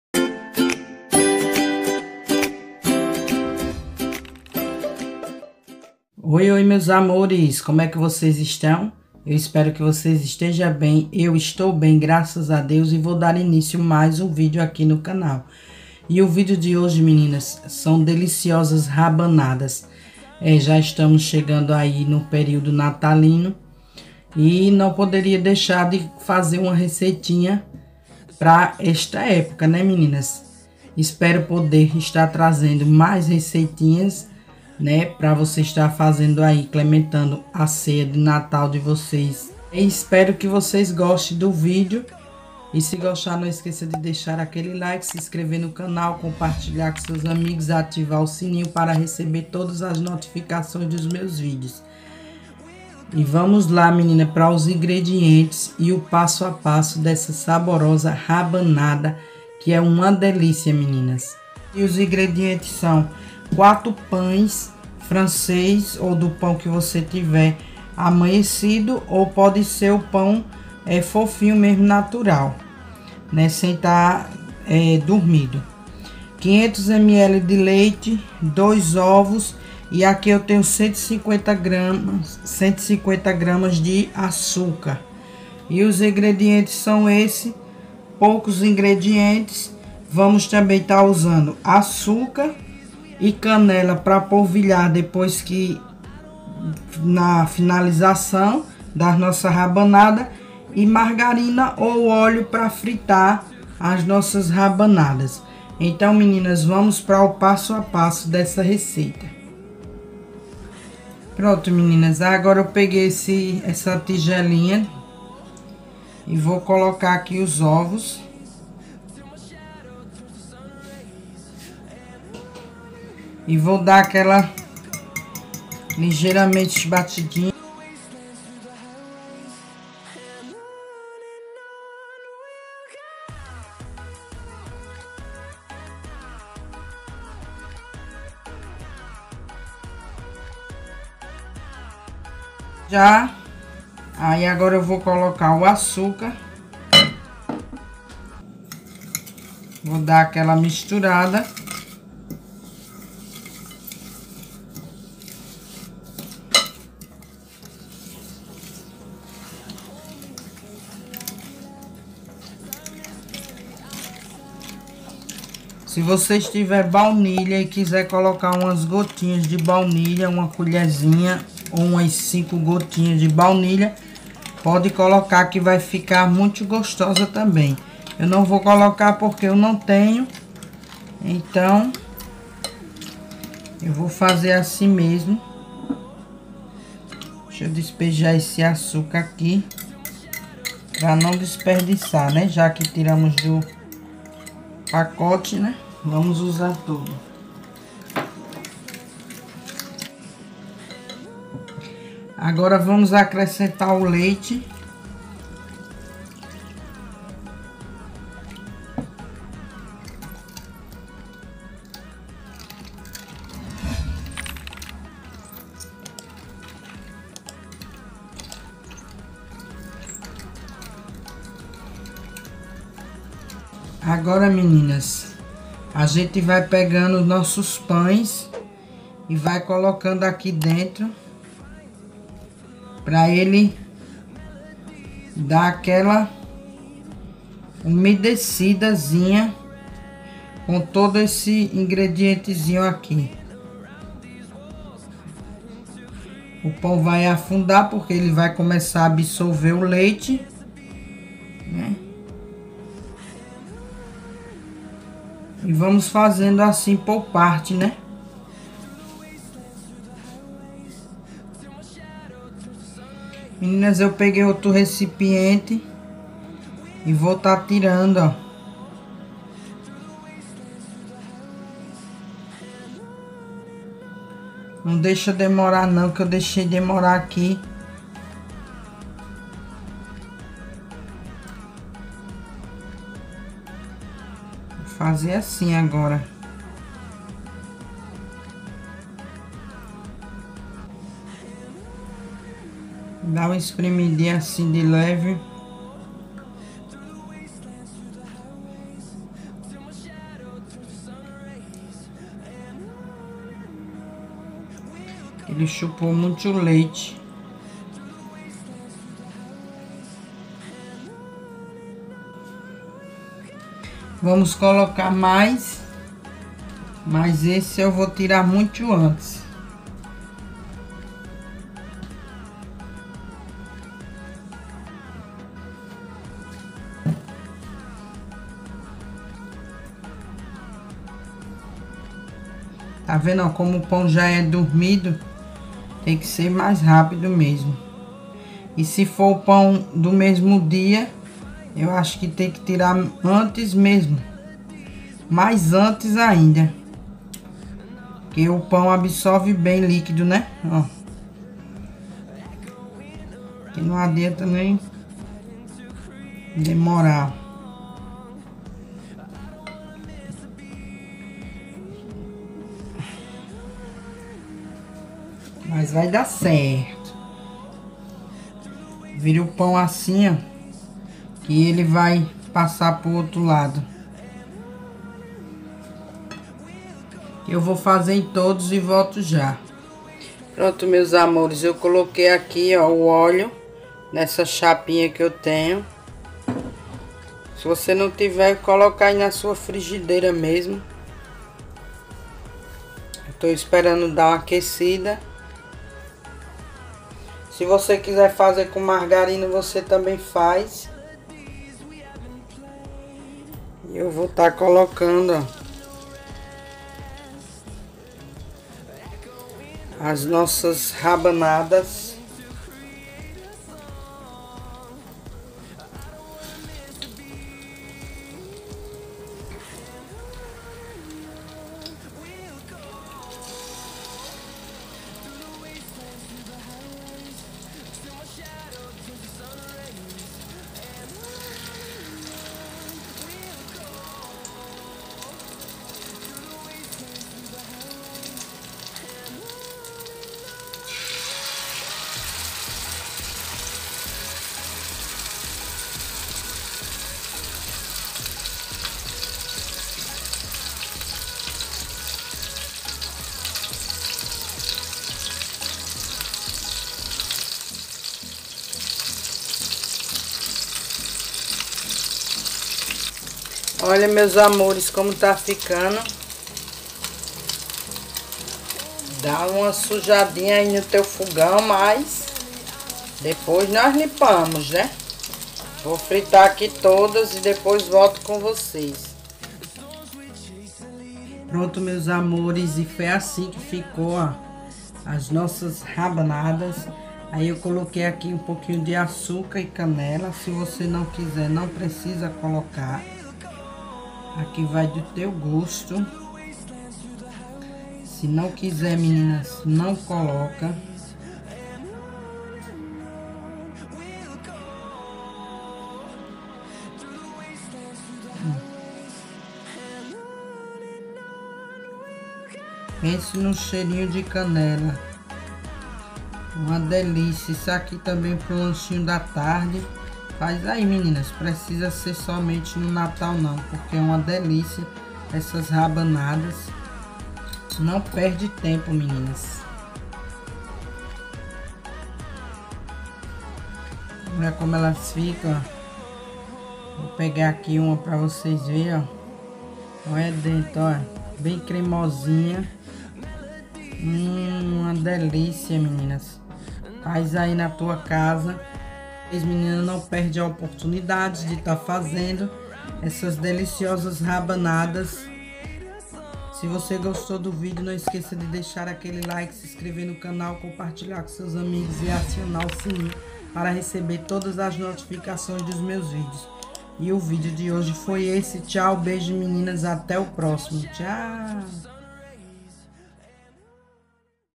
RABANADA(SUPER FÁCIL)RCEITA DE NATAL
oi meus amores, neste vídeo eu
 mostro a vcs essa saborosa receitinha de rabanada espero que tenham gostado beijos da Rose 😘🌹